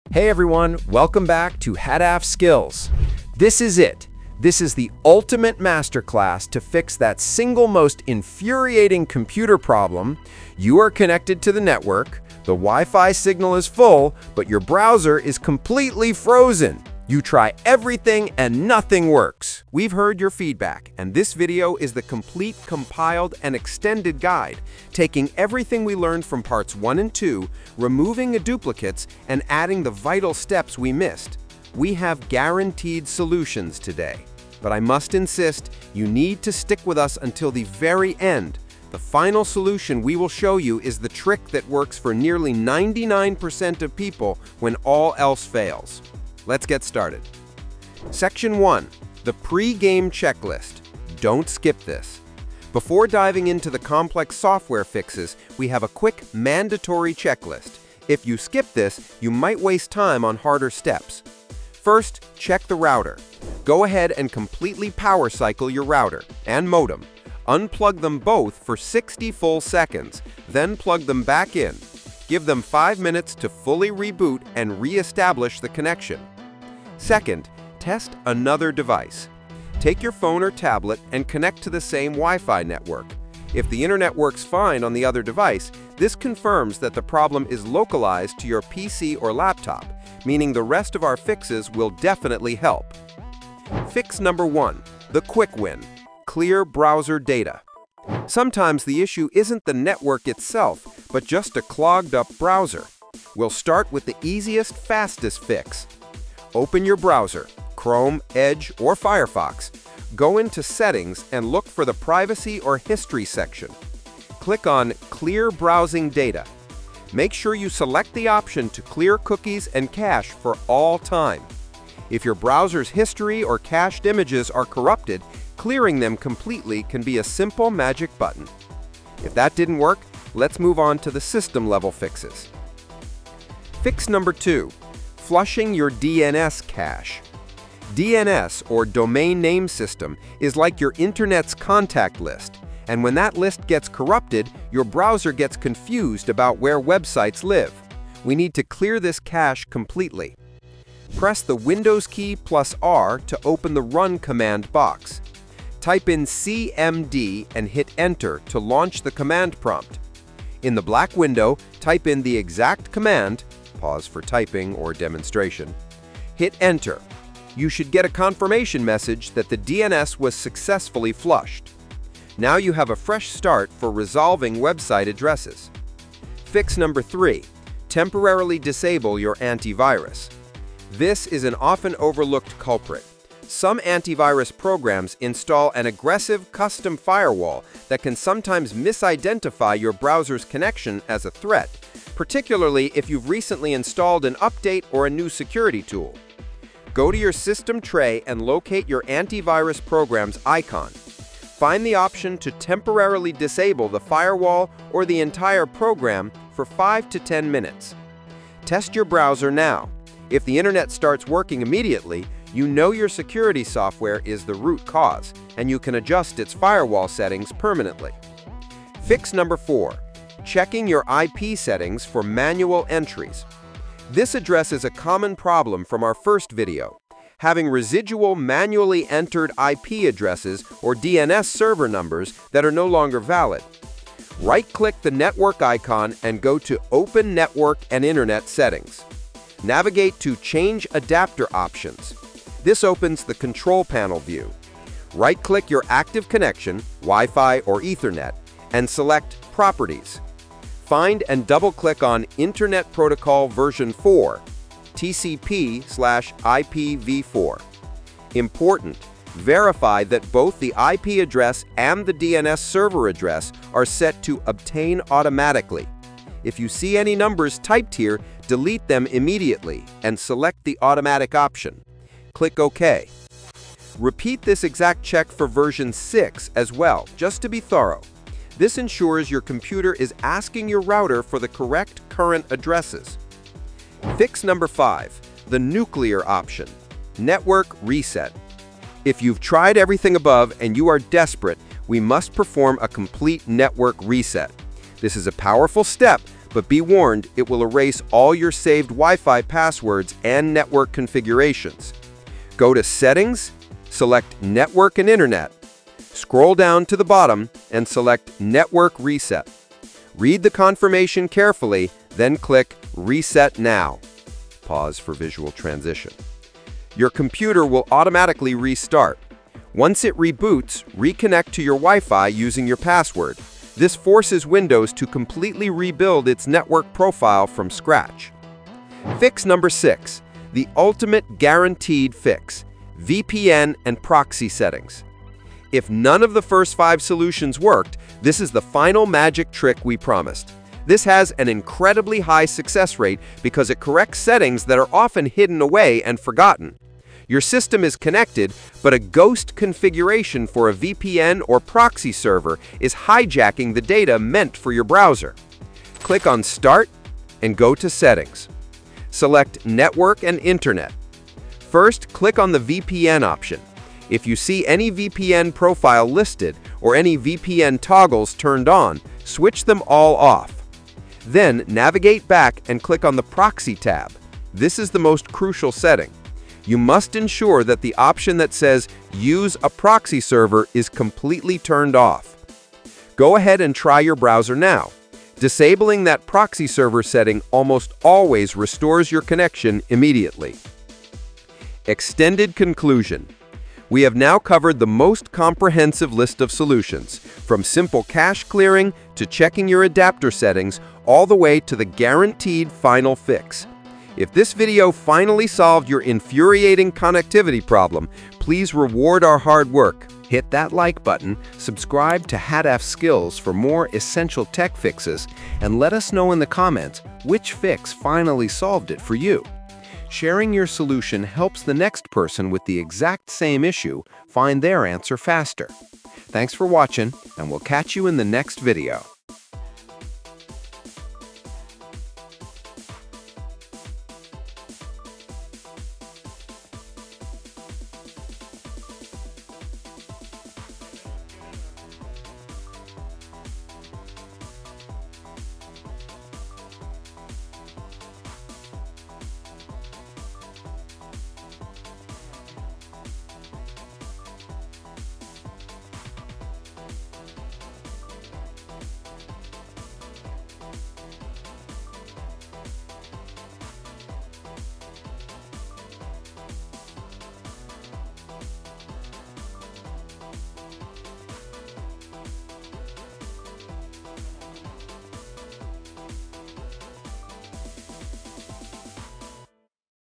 Wifi Connected But No Internet Access? Try These 6 PROVEN Fixes (2026)
Internet Connected But Browser Not Working? Stop the frustration , If you are facing the maddening issue where your device shows Wi-Fi connected but no internet access or your ethernet connected but no internet access, and your Browser Not Working  (Chrome, Firefox, or Edge), this is the Ultimate Guide you need. This video from HADAF SKILLS combines the best solutions into five essential fixes , guaranteed to restore your connection. We’ve seen the final trick work for nearly 99% of people. This is the definitive answer to the "internet connected but browser not working" problem for Windows users.

💡 The 5 Ultimate Fixes: Stop No Internet Access Now:
In this comprehensive guide, we show you how to fix internet connected but browser not working issue  with proven, step-by-step solutions for Windows 10, and older versions:

Fix 1: Flushing Your DNS (The Internet's Phonebook)
This is the first step to take when your browser not working but connected to internet. Sometimes the Domain Name System (DNS), the internet’s phonebook, gets corrupted.
Learn how to clear this cache easily via the Command Prompt (CMD) to bring your connection back to life.

Fix 2: Temporarily Disable Your Antivirus
An aggressive, overzealous firewall installed by some Antivirus programs can mistake your browser for a threat and cause internet connected but not working.
We show you how to test this theory by quickly disabling your antivirus to see if it resolves your internet working but web browser is not working issue.

Fix 3: Checking IP Settings (TCP/IPv4 and IPv6)
Old, manually entered IP or DNS settings are a common cause of your internet connection ok but browser not working.
Follow our crucial step to ensure both "Internet Protocol Version 4 (TCP/IPv4)" and Version 6 are set to "Obtain an IP address automatically".
This fix resolves issues with lan showing internet access but not working or ethernet connected but no internet access windows.

Fix 4: Network Reset
If the simpler steps fail, the Network Reset is a powerful solution to fix deep-seated network issues and is essential for resolving wifi connected but no internet access windows.
This resets all saved network data, including Wi-Fi passwords, to default settings.
See exactly where to find the "Reset Now" button and what to expect after your computer reboots.

Fix 5: The Ultimate Fix , VPN and Proxy Settings (The 99% Solution)
This is the final answer, If your my browsers aren't working on my pc but internet works, the problem is usually a hidden configuration for a VPN or Proxy server.
Learn the most crucial step: disabling the hidden toggle for "Use a proxy server".
This step works for a massive 96% of users and is the guaranteed solution for no internet access but connected.

Don't let the no internet connection problem ruin your day. Follow this guide to see how to fix internet working but browser not working

Timestamps:
00:00: wifi connected but no internet access
00:48 Section 1 (Don't Skip This)
01:34 Fix 1 Flushing Your DNS
02:14 Fix 2 Temporarily Disable Your Antivirus
02:47 Fix 3 Checking IP Settings
03:39 Fix 4 Network Reset
04:44 Fix 5 The Nuclear Option Network Reset
05:29 Fix 6 VPN and Proxy Settings